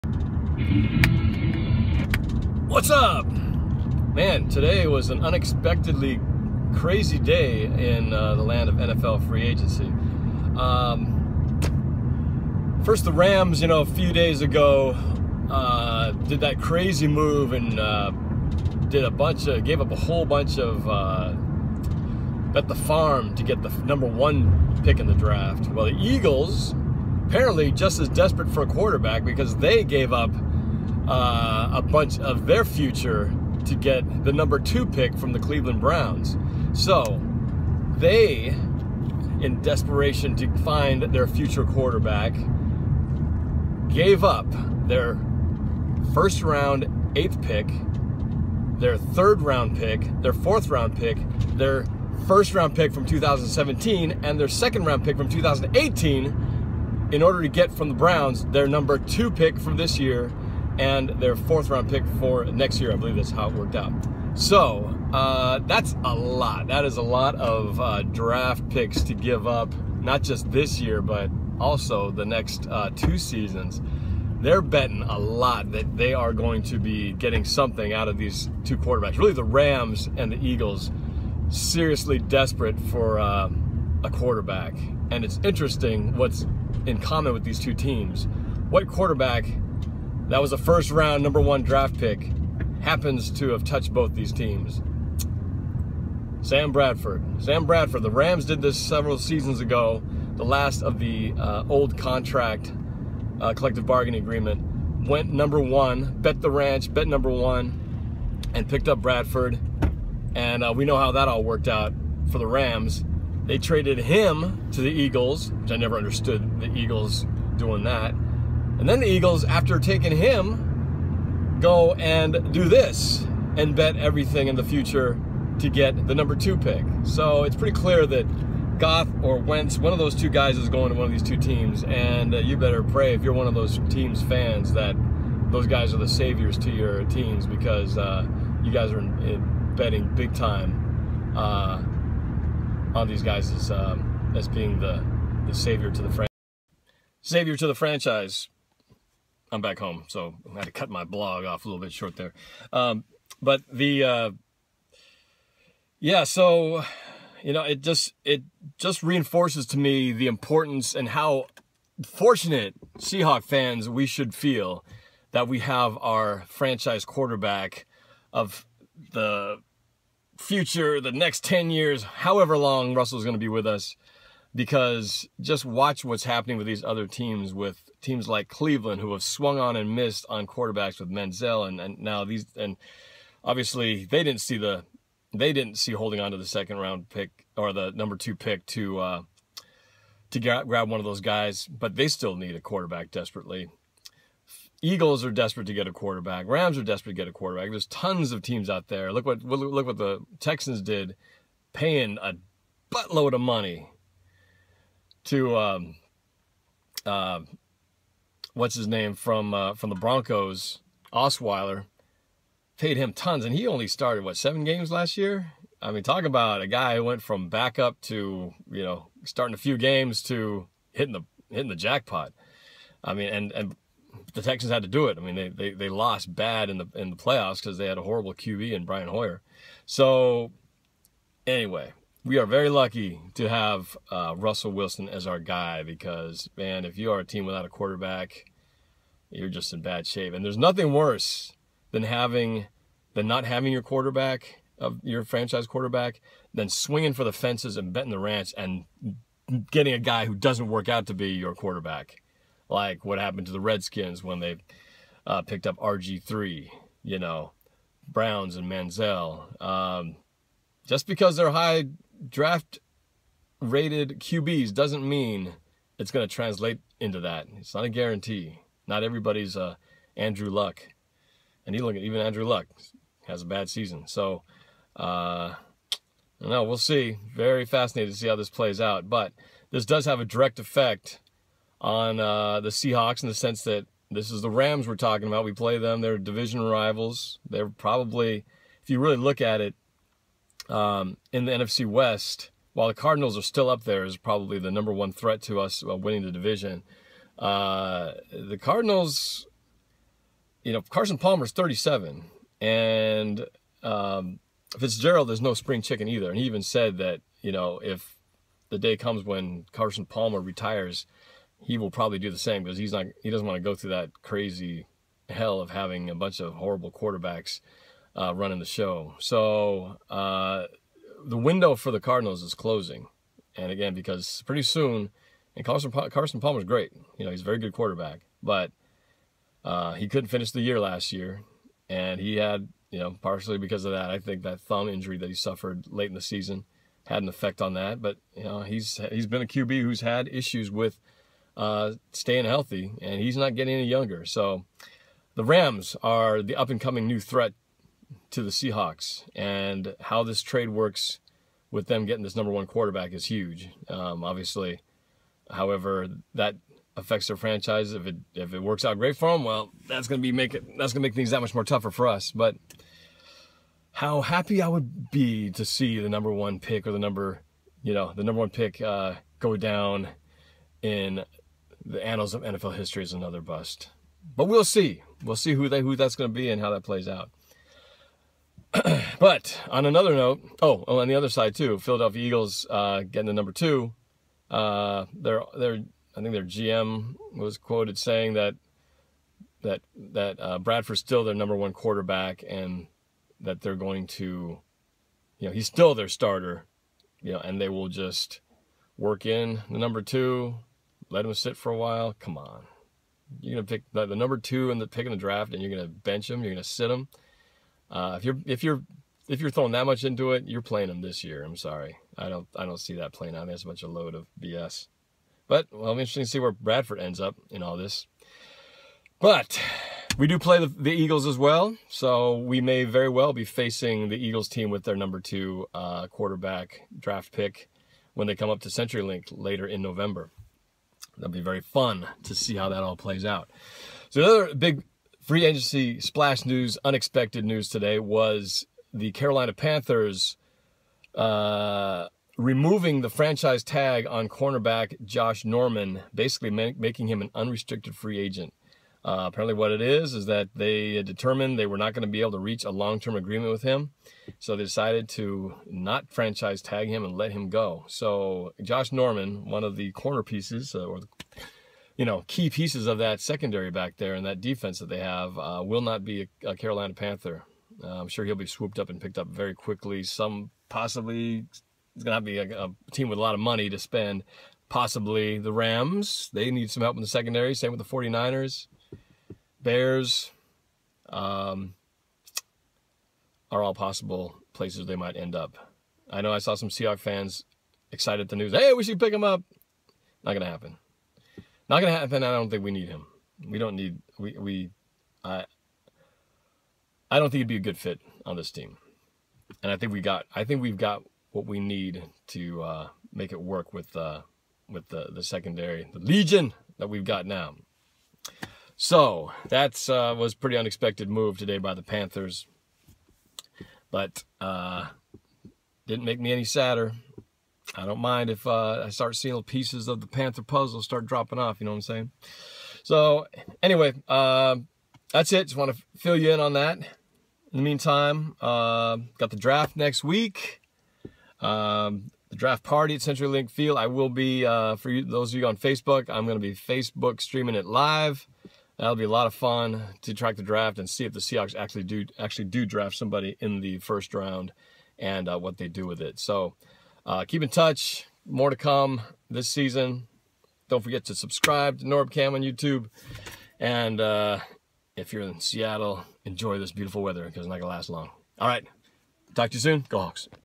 Eagles all in for #2 pick, Panthers release Josh Norman
Rams and Eagles are clearly desperate for a QB. Panthers unwilling to break the bank on CB, Norman. Who's bet will pay off?